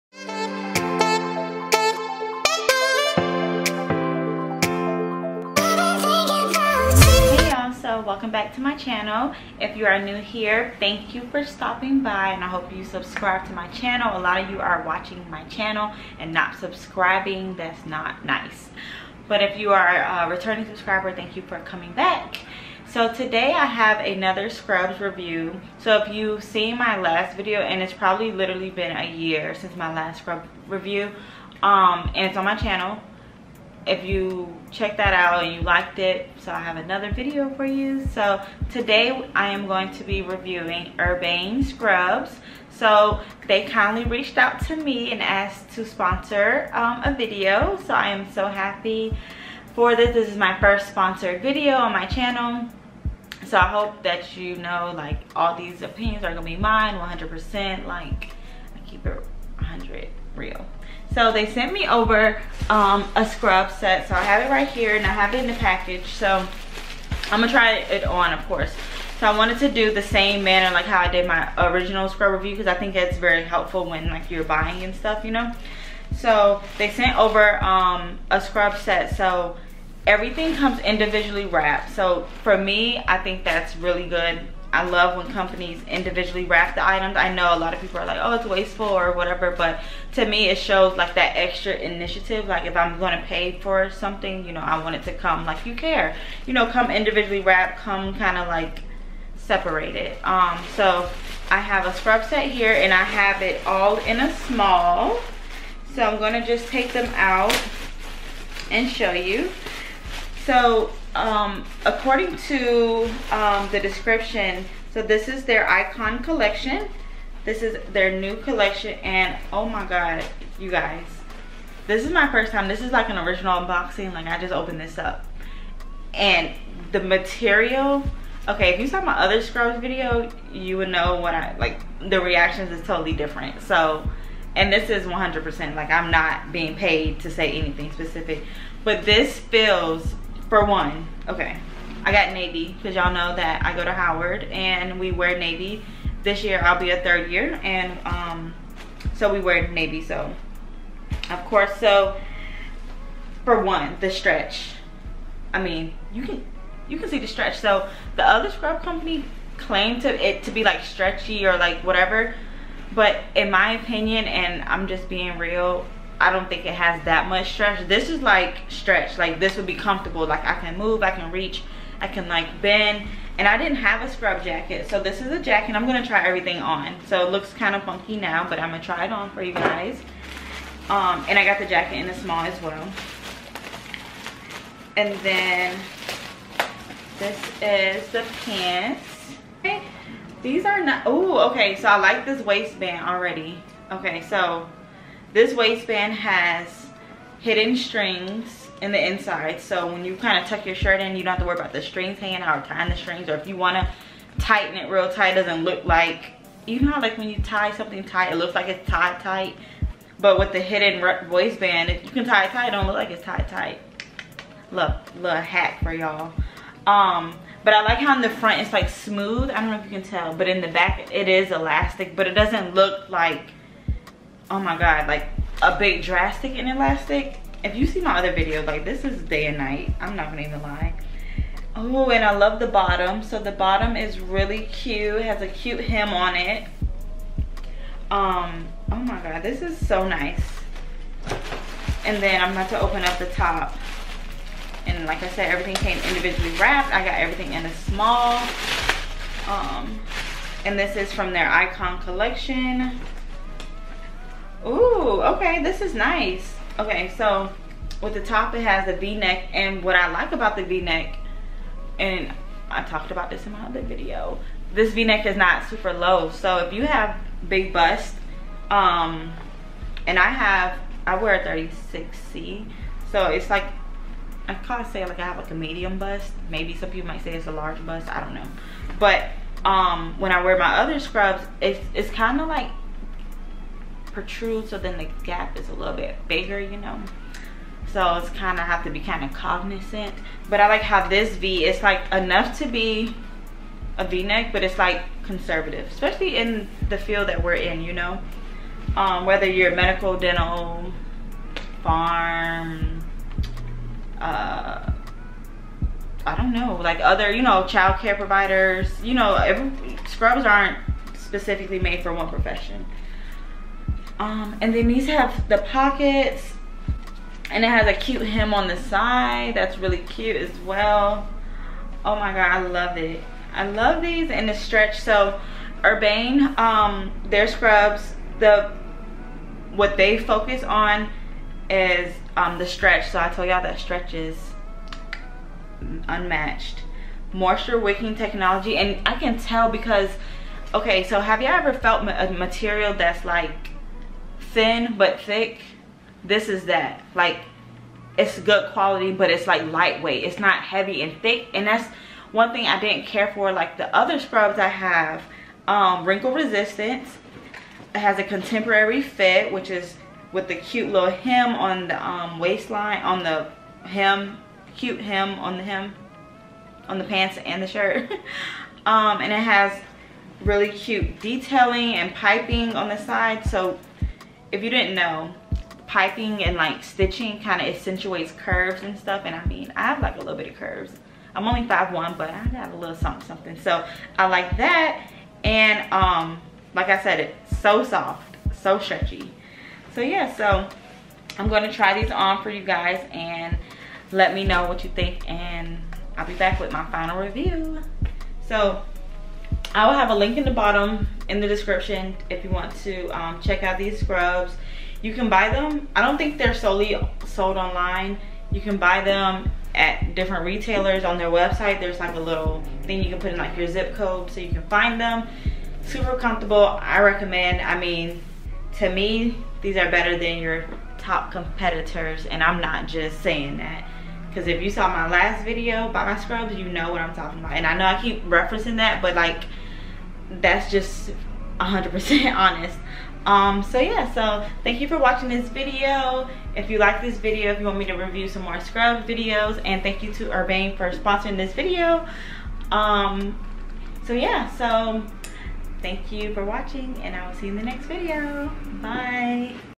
Dental School- I tried Urbane ICON Collection Scrubs and Ya'll......| The Dental League
Hey ya'll,
I tried Urbane Scrubs and ya'll really have to check them out ! All the thoughts are mine of course. !

xoxo Bri 💕

- - Editing: iMovie